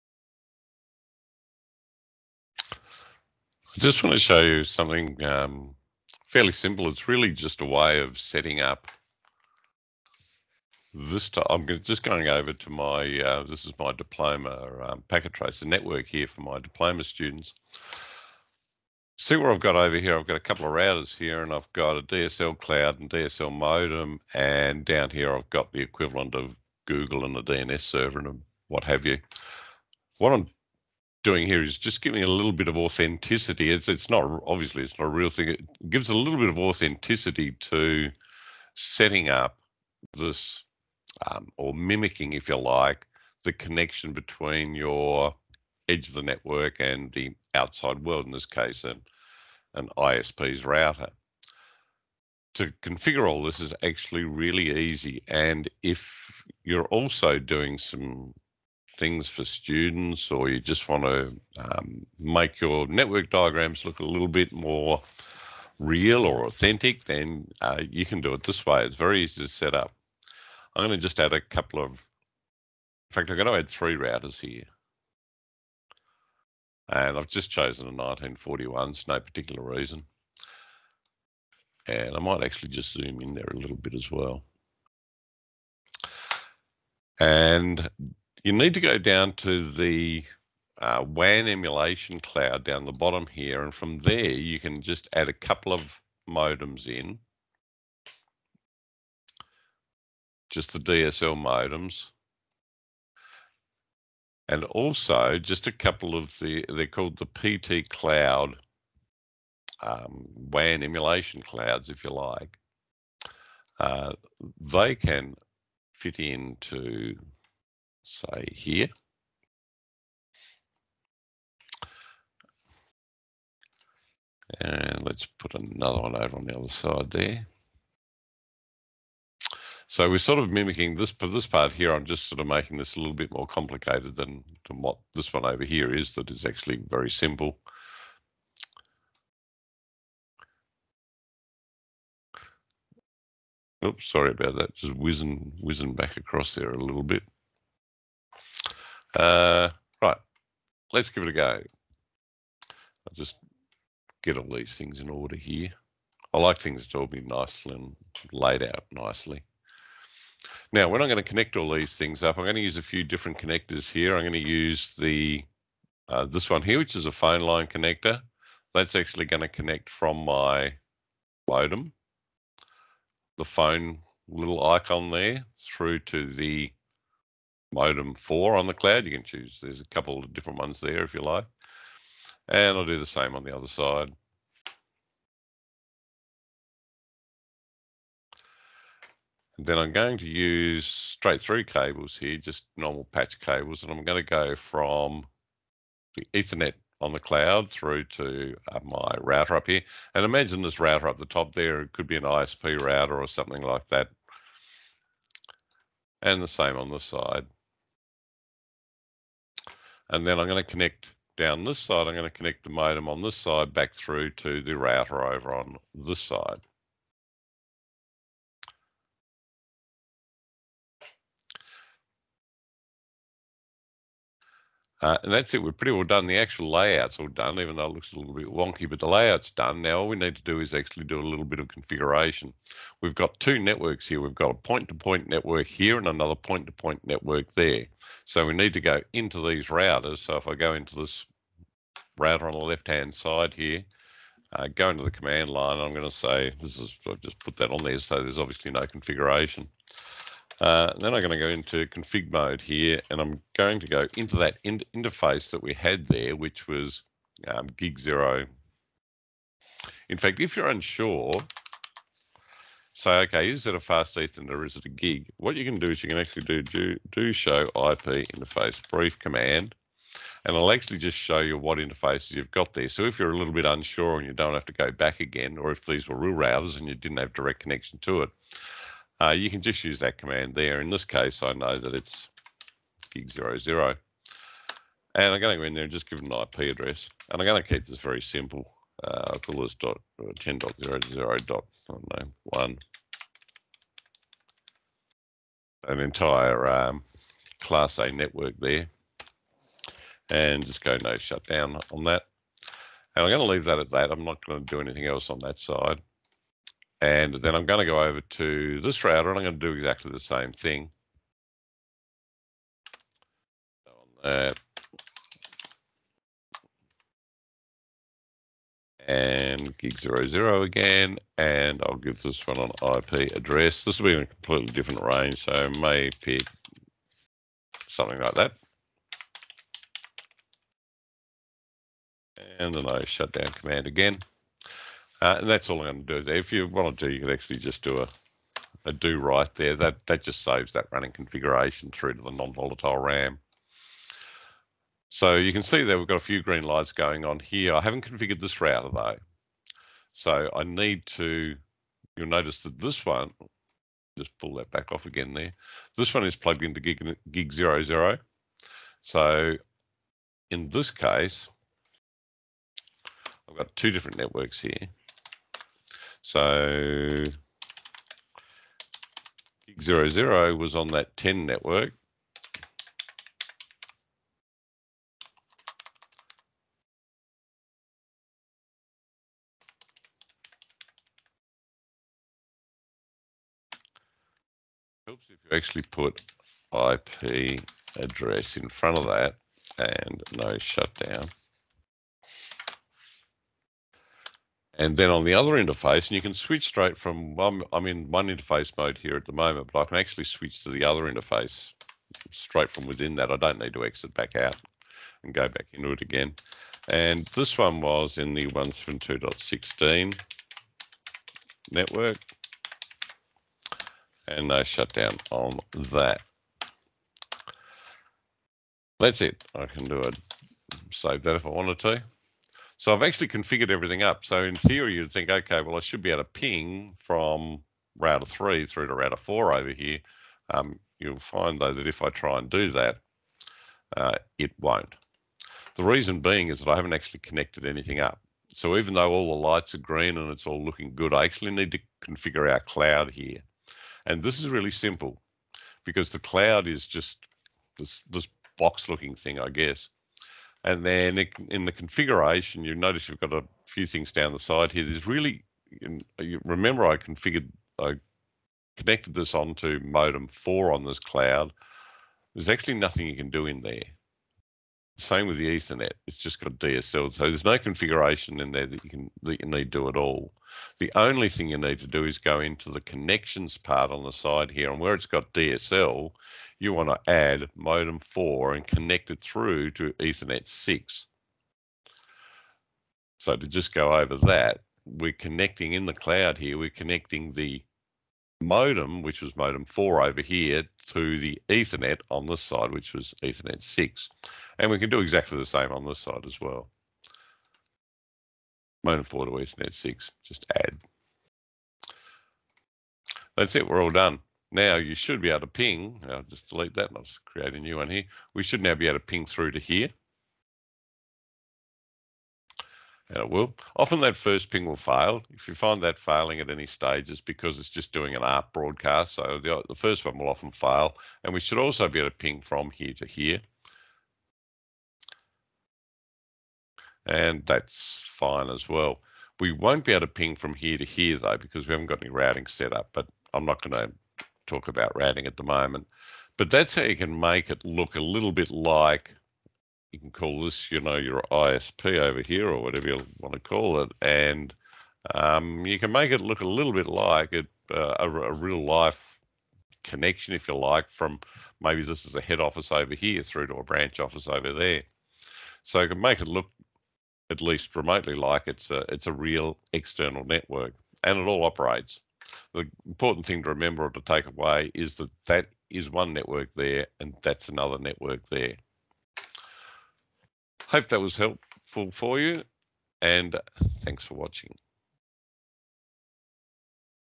Configuring the DSL Modem and WAN Emulation Cloud in Packet Tracer
Mimicking an outside connection from an edge router to an ISP and from there to another edge router that may be at a branch office (or similar). This is done using three Cisco 1941 routers, two DSL modems and two WAN Emulation clouds (PT Cloud). Simple configuration in the routers and establishing the connections in the WAN clouds.